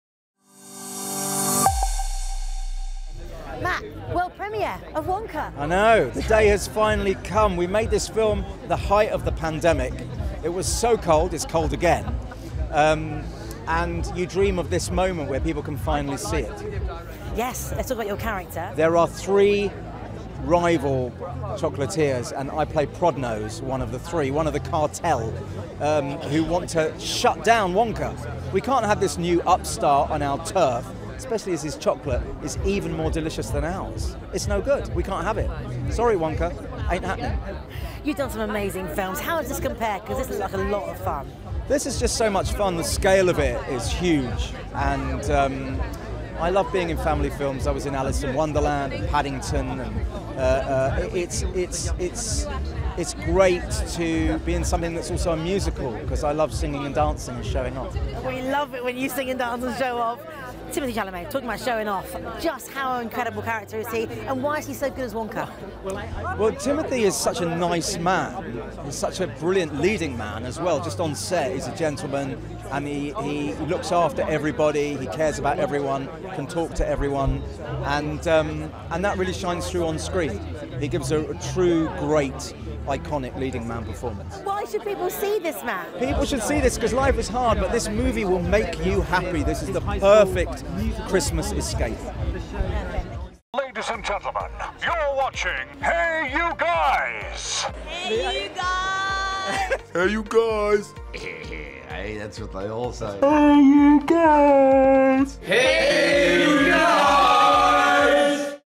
Wonka World Premiere - Matt Lucas Red Carpet Interview
Matt Lucas is interviewed on the red carpet Premiere for Wonka. Directed by ‘Paddington’s’ Paul King, the movie stars Timothée Chalamet, Calah Lane, Keegan-Michael Key, Paterson Joseph, Matt Lucas, Mathew Baynton, Sally Hawkins, Rowan Atkinson, Jim Carter, Natasha Rothwell, Rich Fulcher, Rakhee Thakrar, Tom Davis, Kobna Holdbrook-Smith, with Olivia Colman.

Plot: Based on the extraordinary character at the centre of ‘Charlie and the Chocolate Factory,’ Roald Dahl’s most iconic children’s book and one of the best-selling children’s books of all time, “Wonka” tells the wondrous story of how the world’s greatest inventor, magician and chocolate-maker became the beloved Willy Wonka we know today.

The movie hits cinemas on December 8th.